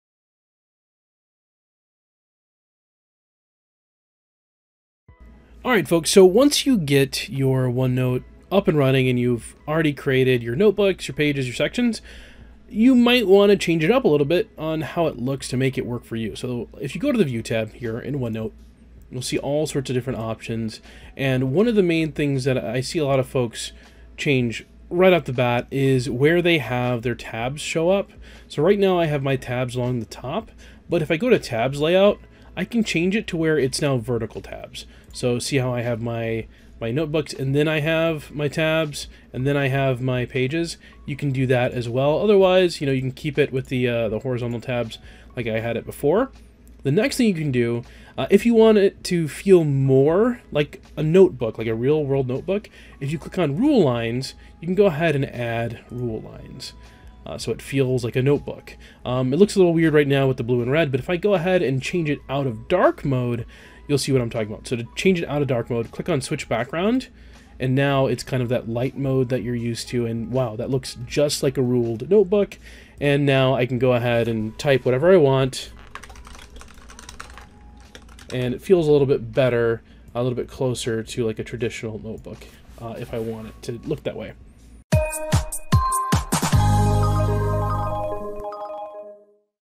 How to Customize OneNote
Check out our helpful tutorial for customizing OneNote in order to improve the effectiveness of your notetaking capabilities.